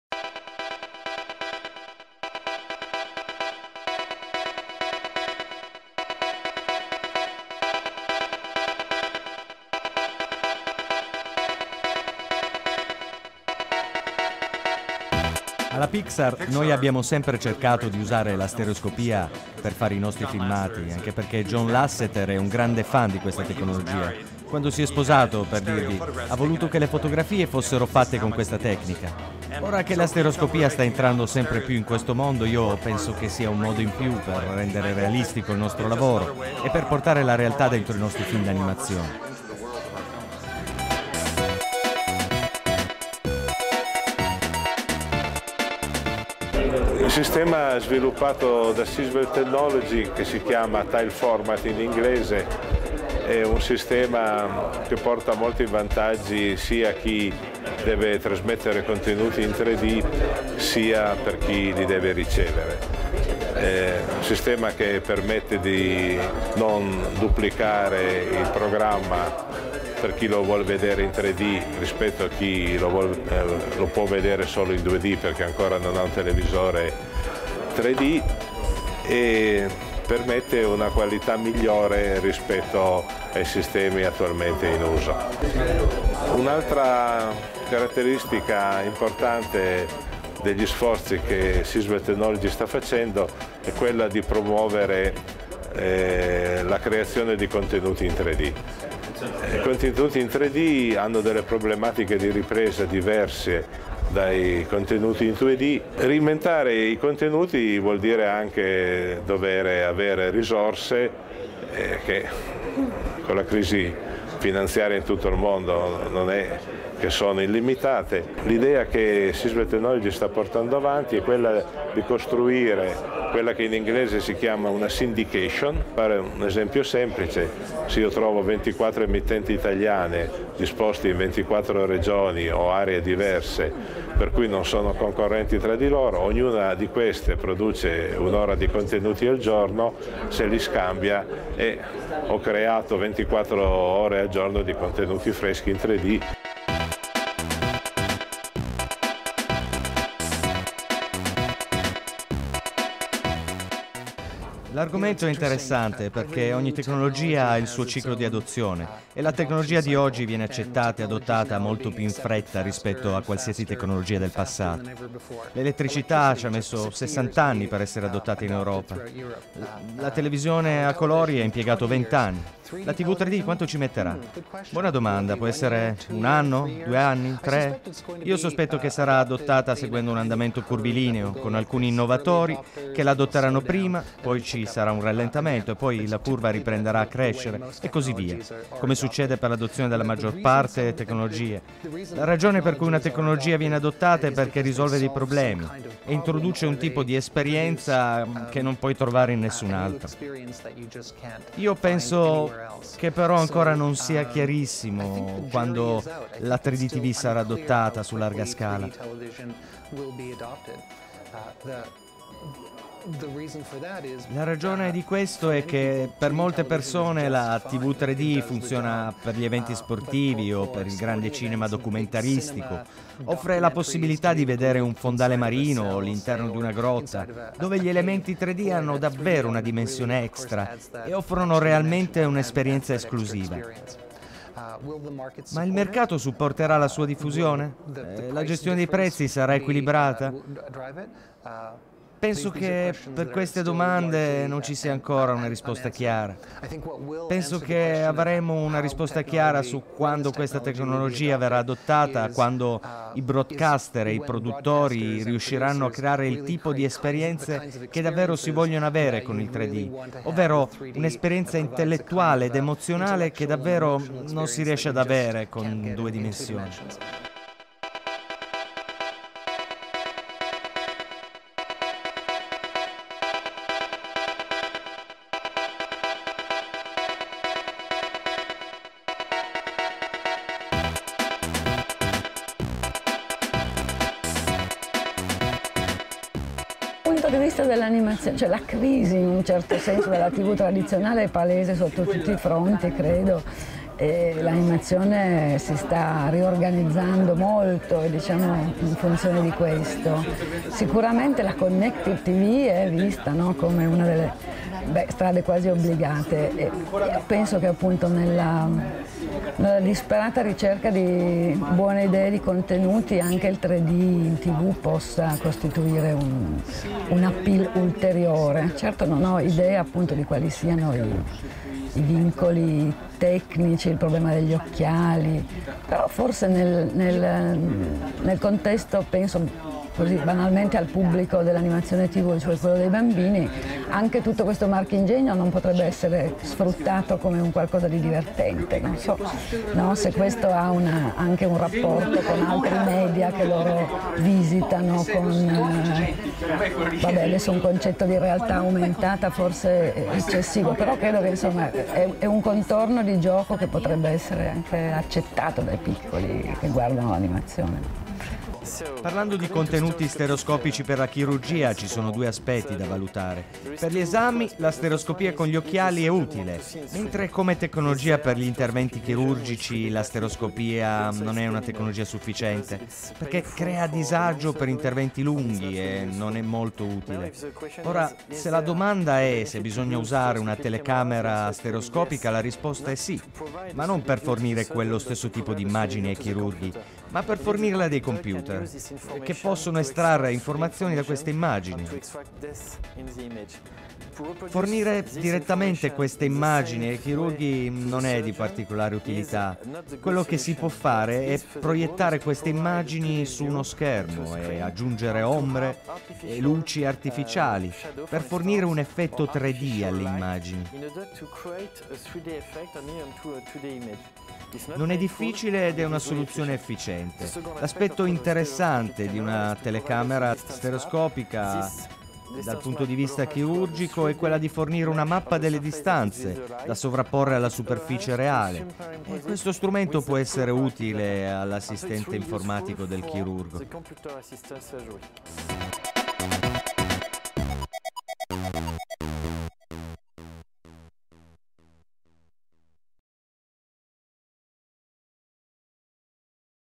View Conference 2011 - Opinioni sulla 3DTV
Alla View Conference 2011 di Torino abbiamo raccolto alcune opinioni sul futuro della 3D TV e delle applicazioni stereoscopiche in ambito professionale.
Roger Gould della Pixar è molto entusiasta sul tema della stereoscopia mentre Roberto Dini di Sisvel ci spiega cos'è il tile format e come è possibile mettere in piedi sistemi di syndication per la raccolta di contenuti 3d per i broadcasters. Tom Wujec della Autodesk ragiona su come e quando la 3dTV potrà prendere piede sul mercato, mentre Chiara Magri vede un futuro sui contenuti stereoscopici legati al mondo dell'animazione soprattutto per una forma di intrattenimento domestico dedicata ai bambini. Infine Luc Soler, chirurgo ed esperto di informatica applicata alla medicina ci spiega come il 3d e la stereoscopia possono essere utili agli assistenti informatici dei chirurghi.